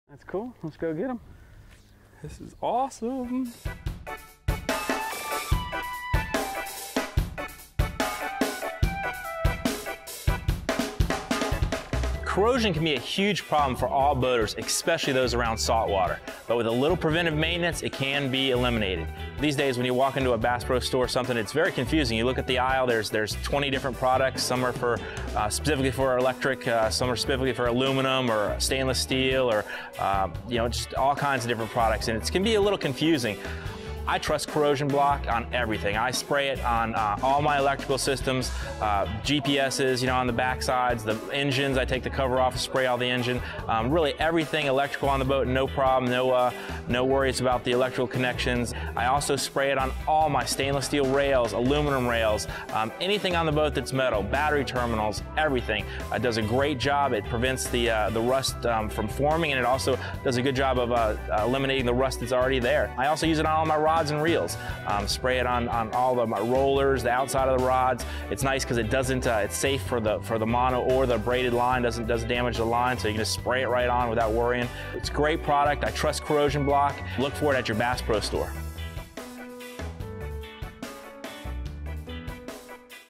How to Prevent Saltwater Corrosion
Corrosion can be a huge problem for all boaters, especially those around saltwater. With a little preventive maintenance it can be eliminated.

If you live anywhere near Saltwater, you need to use Corrosion Block on Everything